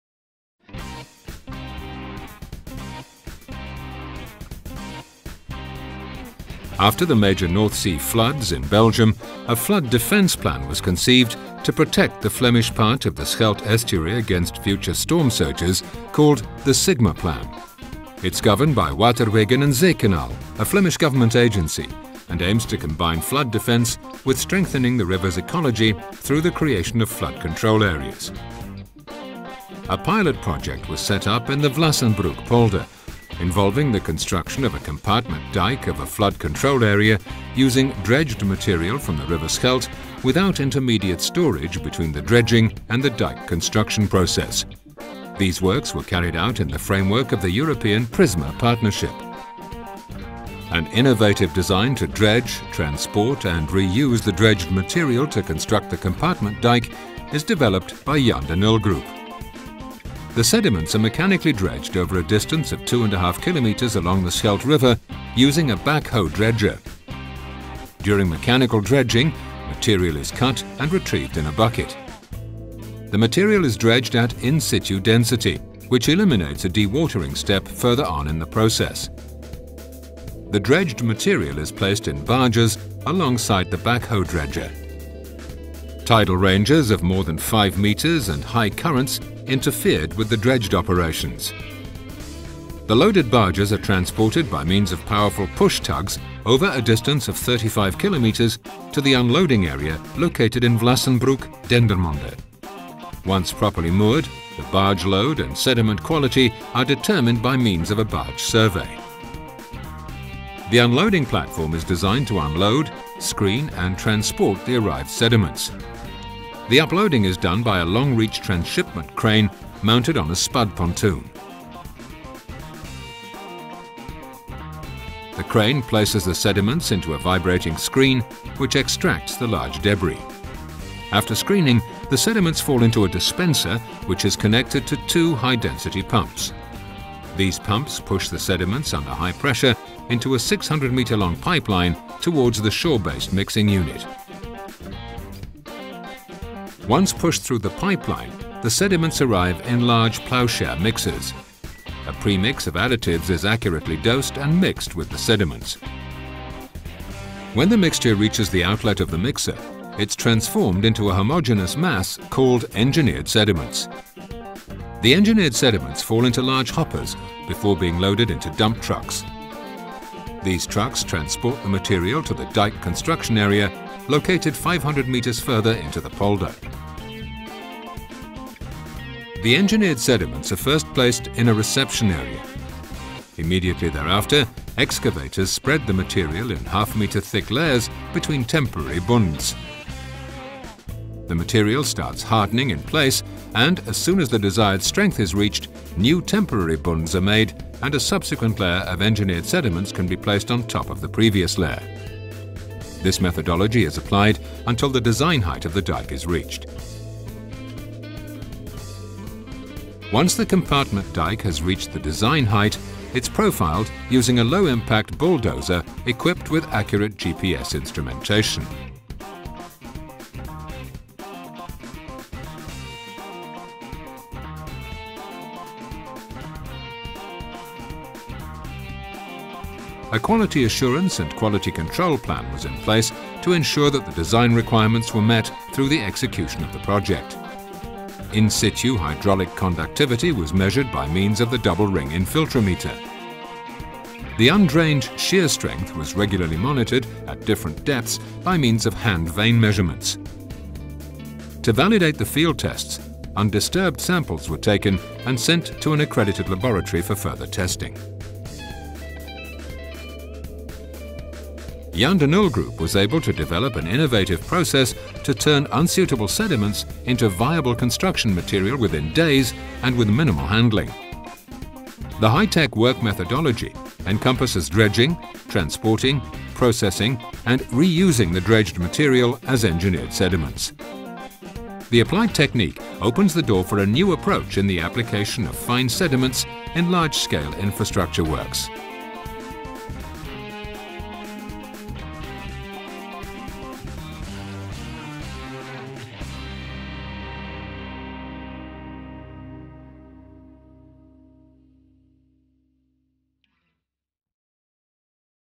Jan De Nul Group - Construction of a compartment dike in Vlassenbroek (Belgium)
The construction of a compartment dike in Vlassenbroek is the first phase in the construction of a controlled flood plain in Dendermonde. Jan De Nul -- Envisan elaborated an innovative technique to construct the dike core with engineered sediments. This technique was geotechnically analysed and modelled in detail. Simultaneously, an elaborate test campaign was set up, examining on lab scale the functioning of 20 potential additives in different concentrations. With a selection of additives, pilot studies were also carried out. Based on the results of these studies, a final design was drawn up and then verified by independent design and engineering offices. The sediment comes from the Zeeschelde between Melle and Gentbrugge. It is brought in over water and dumped by means of an ingenious system based on concrete pumps. The additives are added near the site, before the material is inserted in the dike profile.